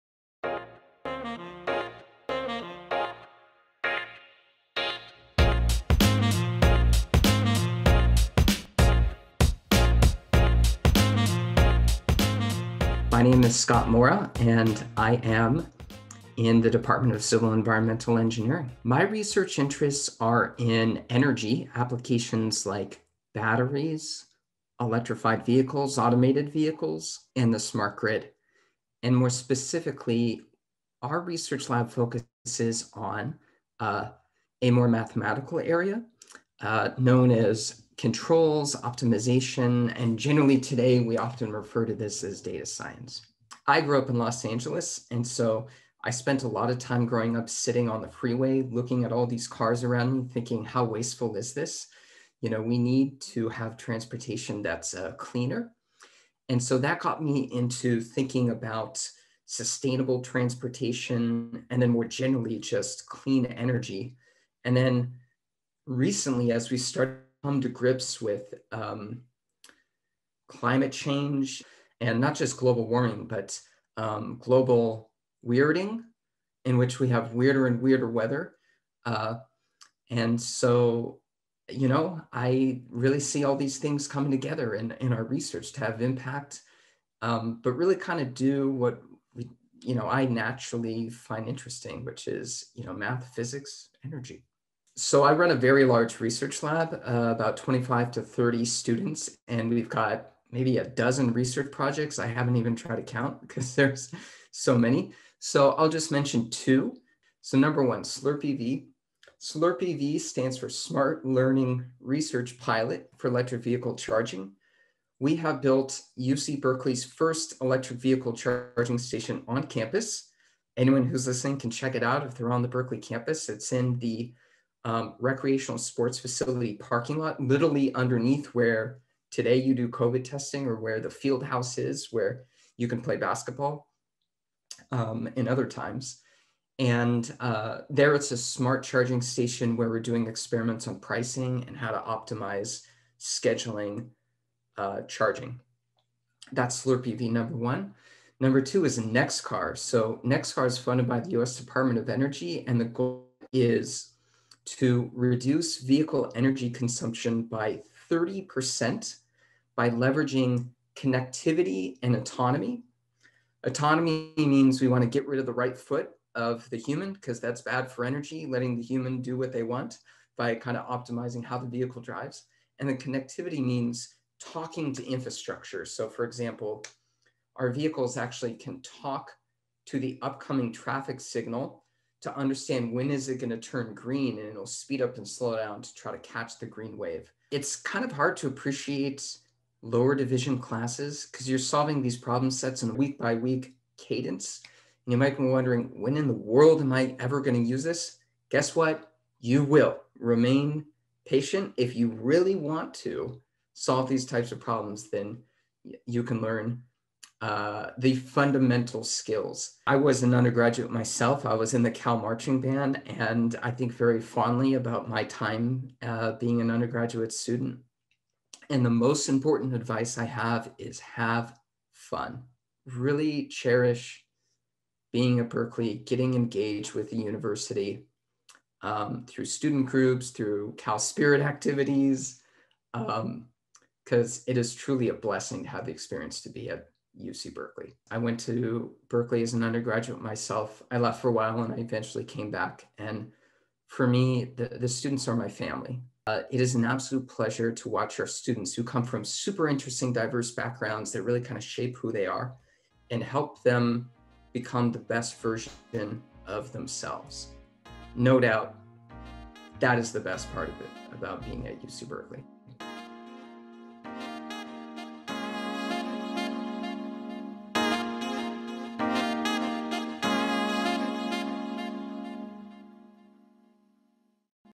Faculty Profiles: Professor Scott Moura
Professor Scott Moura runs the energy, controls, & applications research lab (eCAL). Some of Professor Moura’s research involves clean energy, batteries, and sustainable transportation.